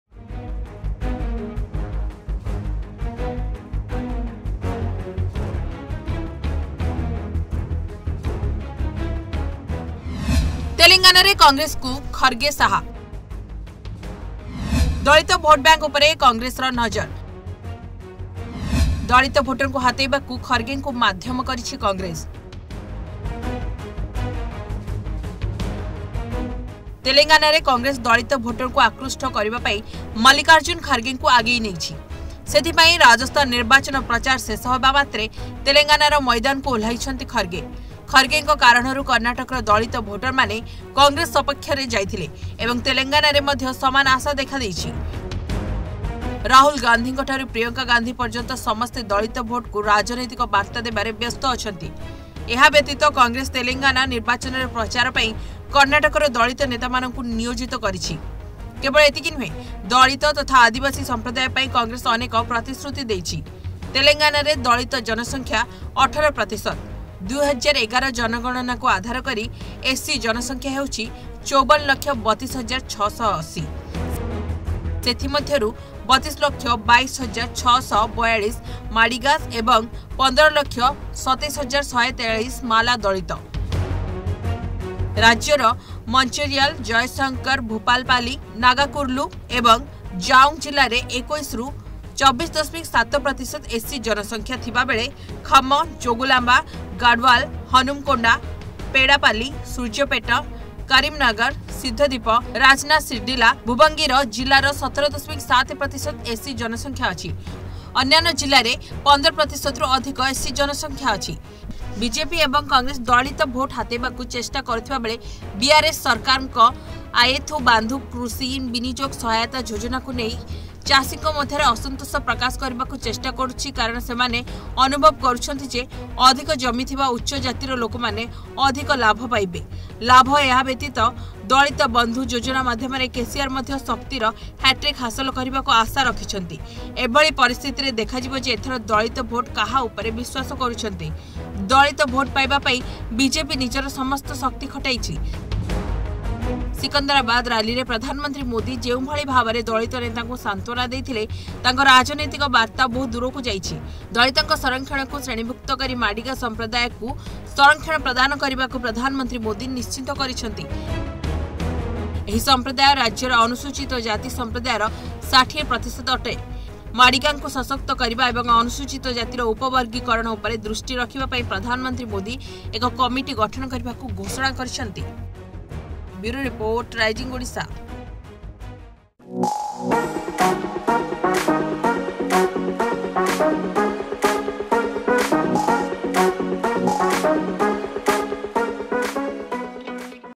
Congress President Mallikarjun Kharge's 'Powerplay' in Telangana Elections; Eyes On Dalit Voters!
Congress President Mallikarjun Kharge's 'Powerplay' in Telangana Elections.